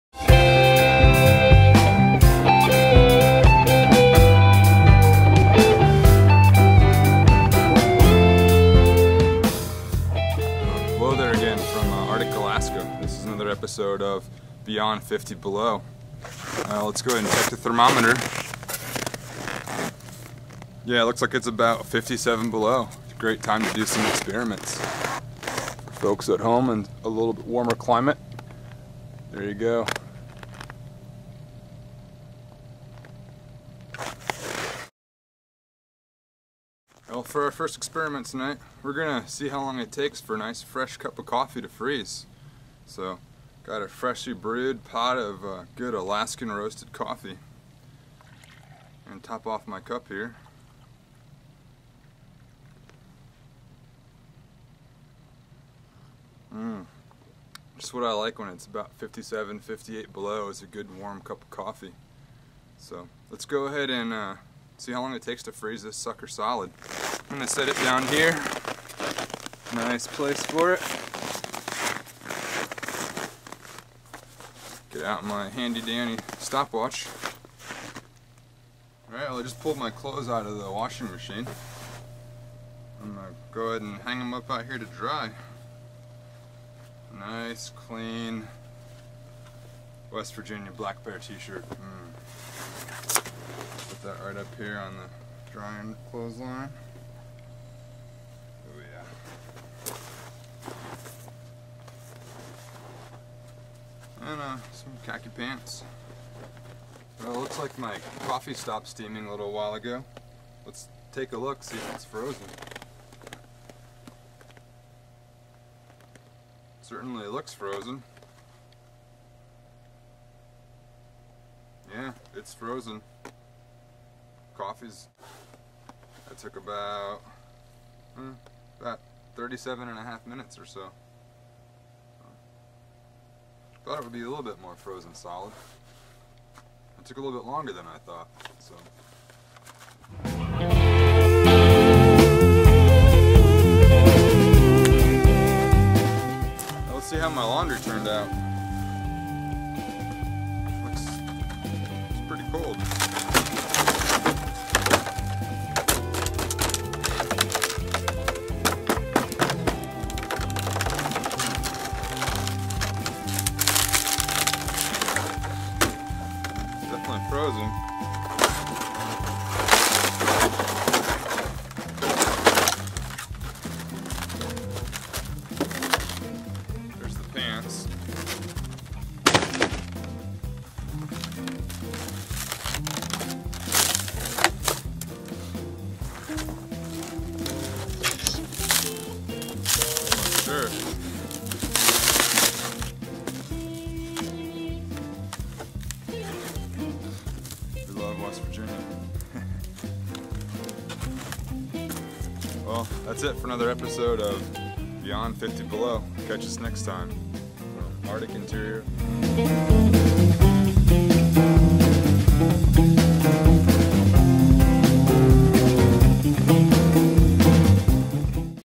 Beyond Fifty Below (Episode 3)
How long will hot coffee last before it freezes?  Check out this latest episode from arctic Alaska.